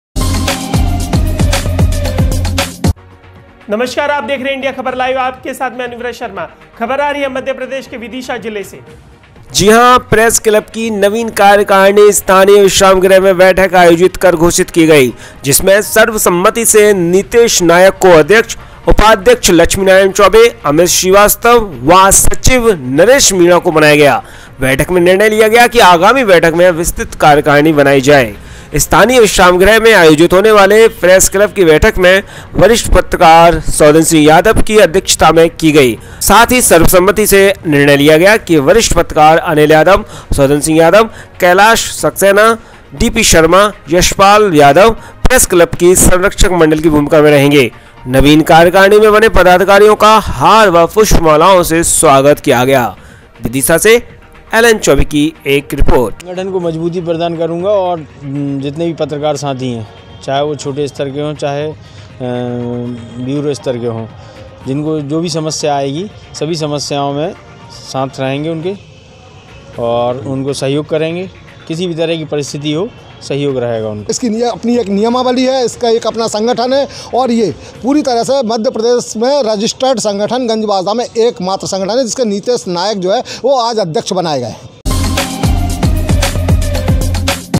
गंजबासौदा प्रेस क्लब की नवीन कार्यकारणी घोषित | इंडिया ख़बर LIVE
विदिशा से संवाददाता एल.एन. चौबे, की रिपोर्ट